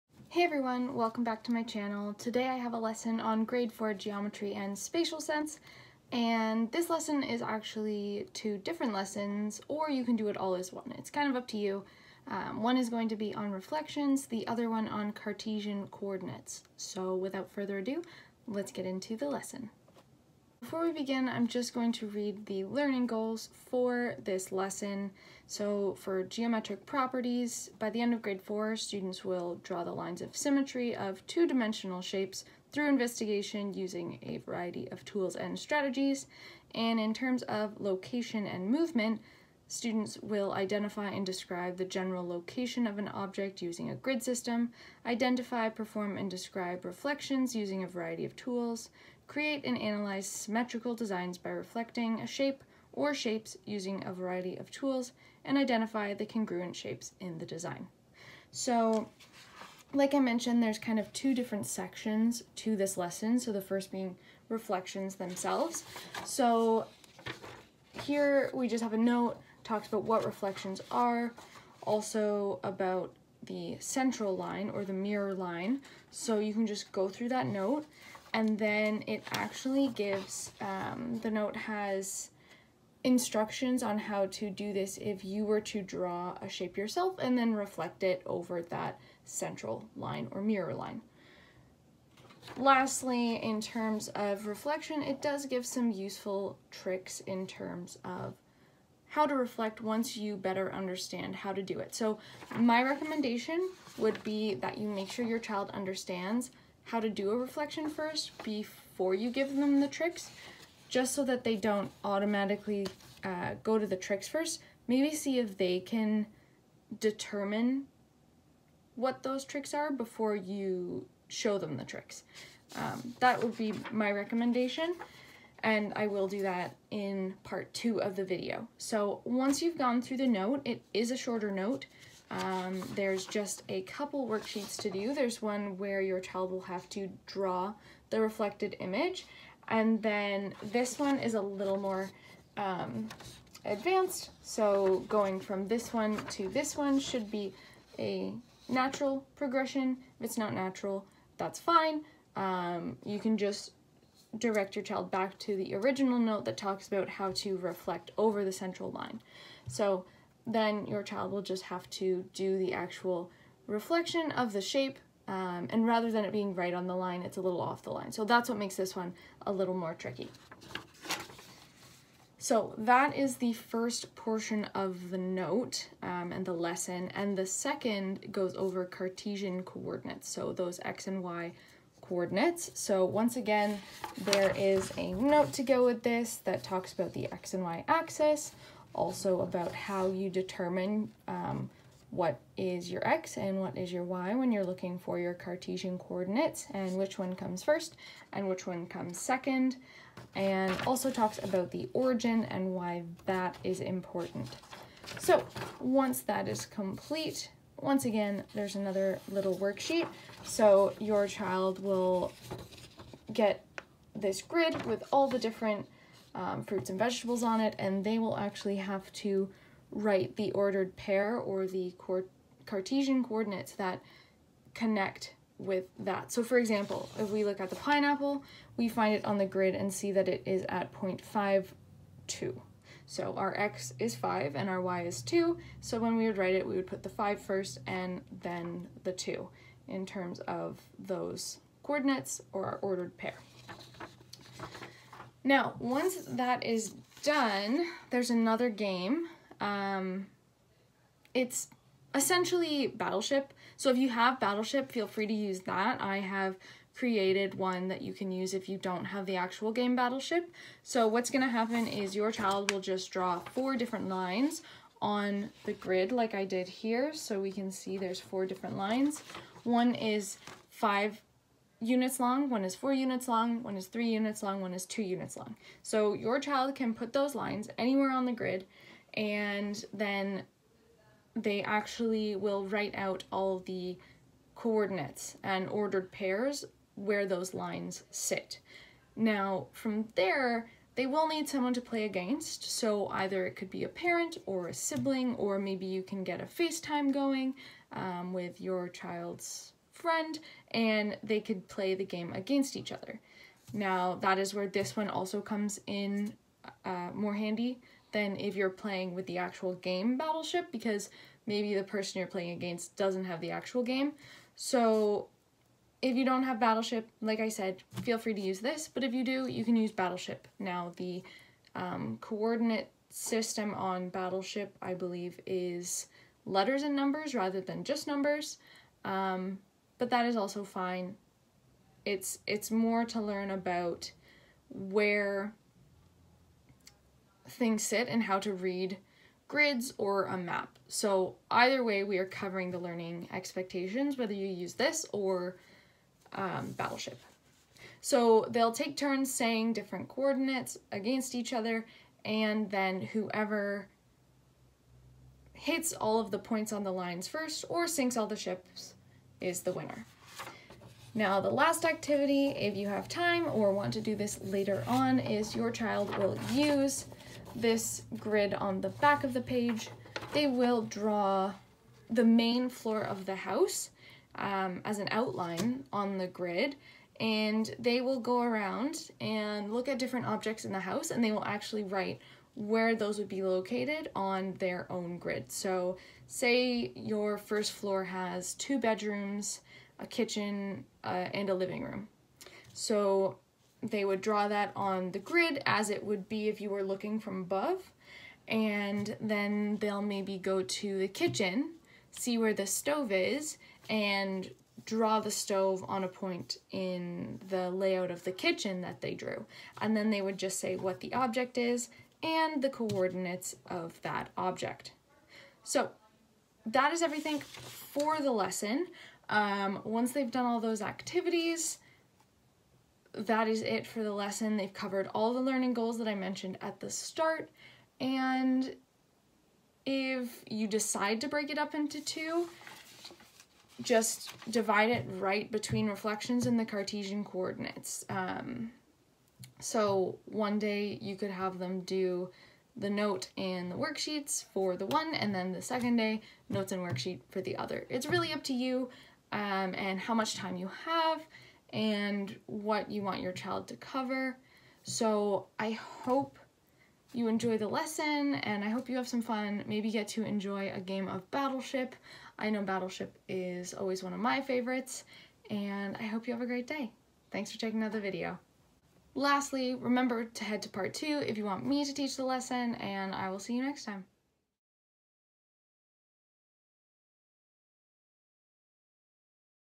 Grade 4 Geometry and Spacial Sense Part 1
This lesson has two different parts that can either be done together or separately.  The two different topics in this lesson are reflections and cartesian coordinates.  I hope you enjoy.